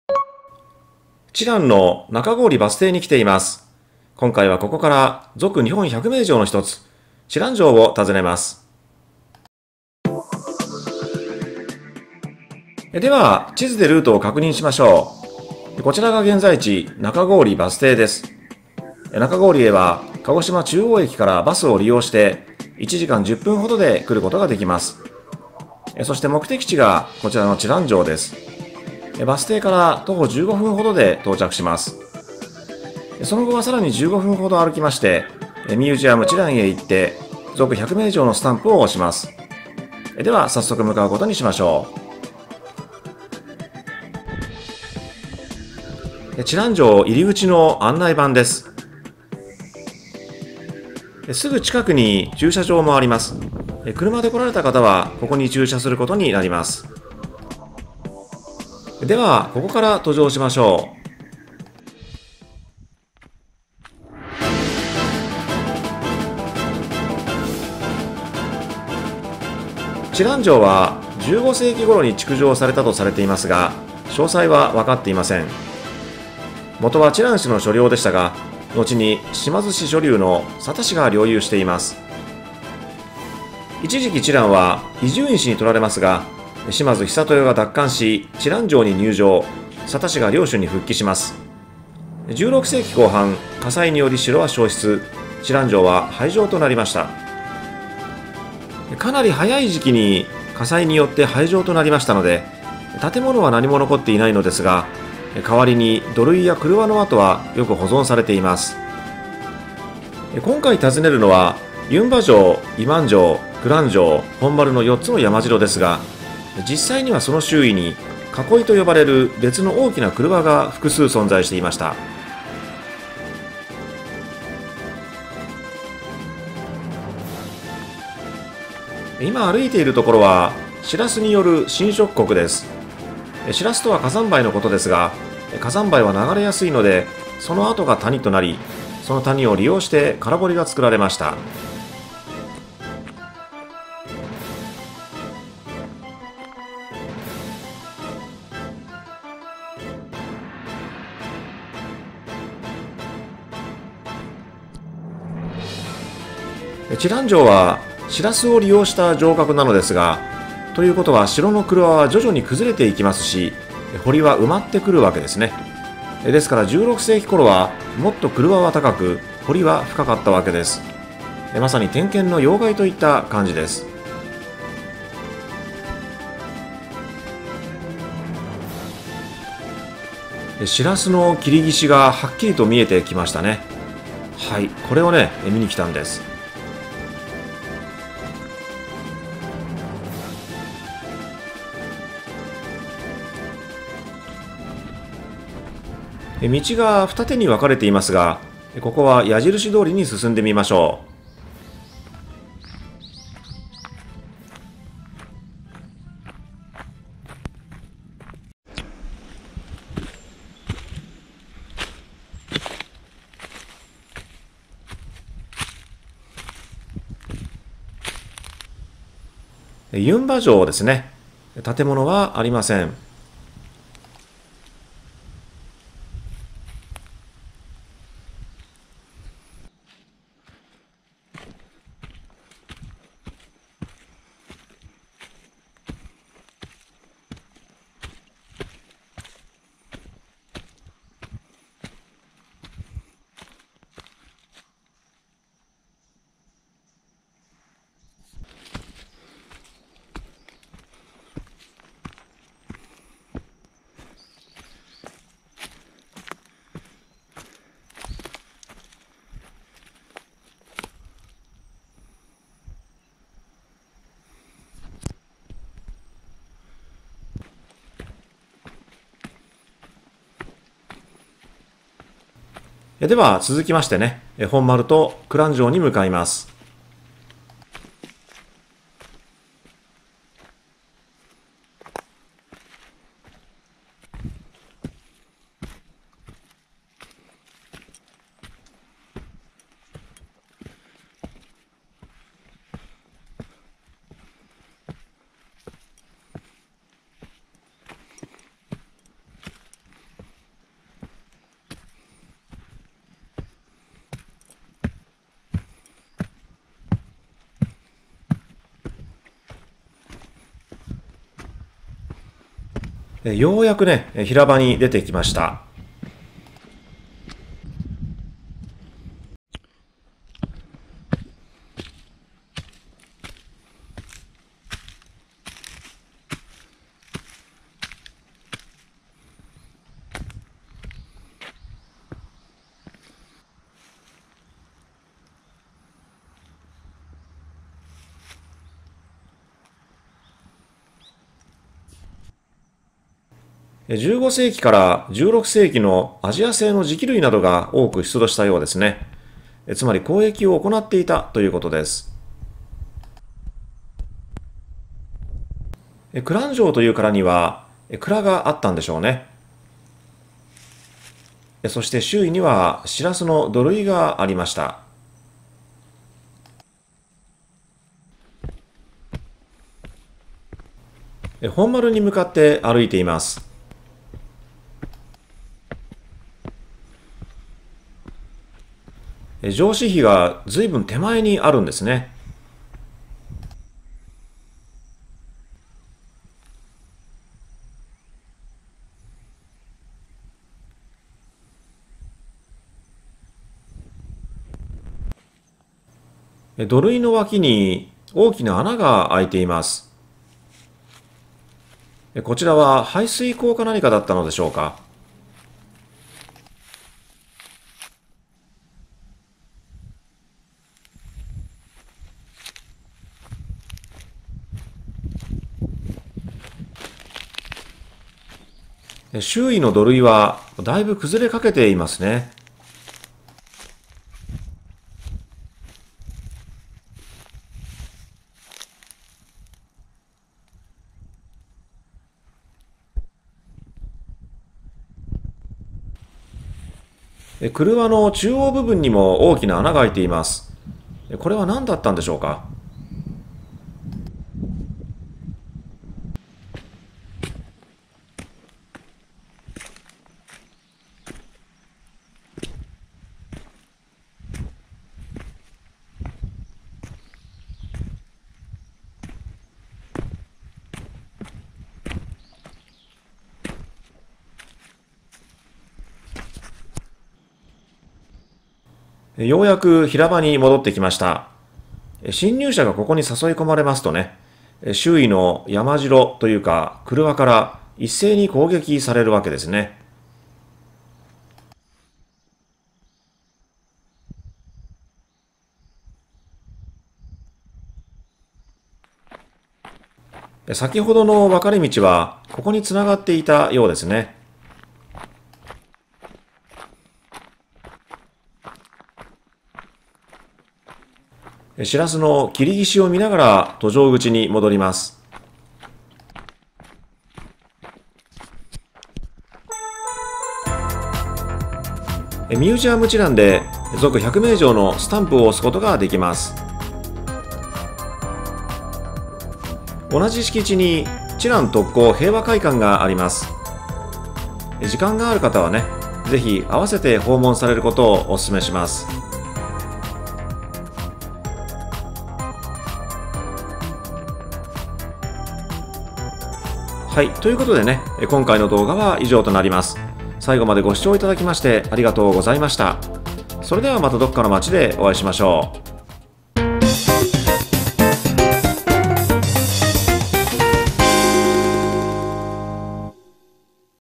【城めぐり】このシラス崖を見に来た 知覧城 鹿児島県【攻略ルート】
★城をひたすらめぐる動画★
シラスが作る大空堀を堪能する

ルート：中郡（バス停）-知覧城（弓場城・今城・蔵之城・本丸）-ミュージアム知覧-知覧平和公園

訪問日：2020/10/13

ジングル：MusMus
地図：Googleマップ
※字幕対応しています。画面の【CC】アイコンでON/OFFを切り替えられます。